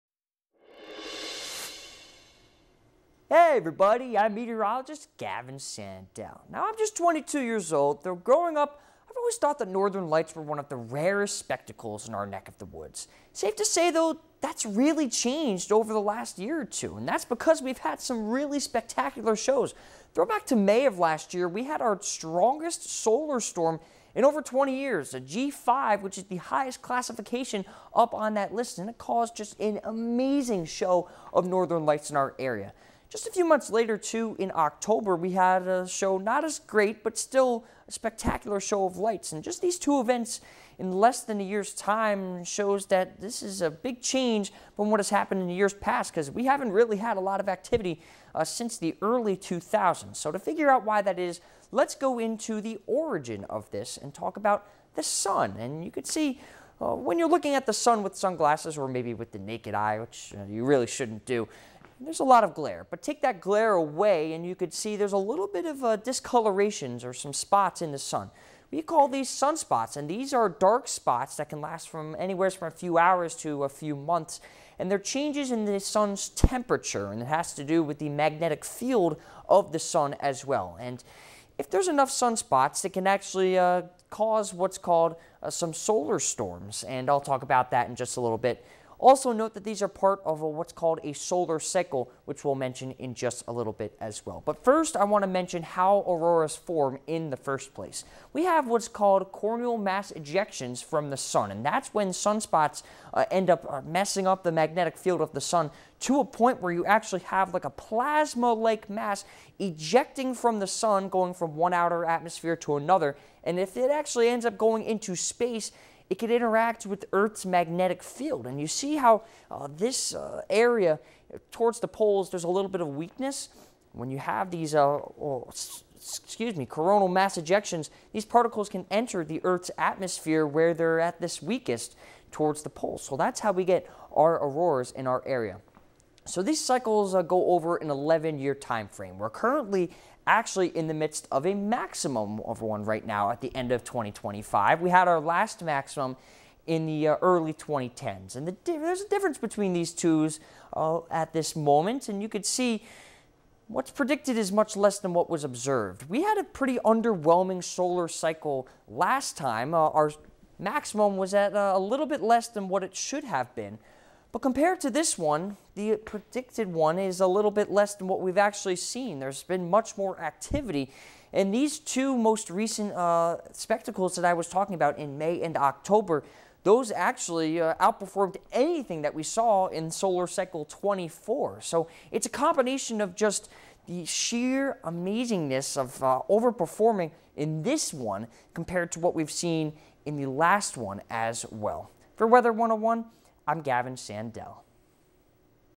Weather 101: Why have we seen the Northern Lights so often?
Since the start of last year, folks around the Valley have been incredibly spoiled to see the Northern Lights in the nighttime sky. Here's why they've gotten so common.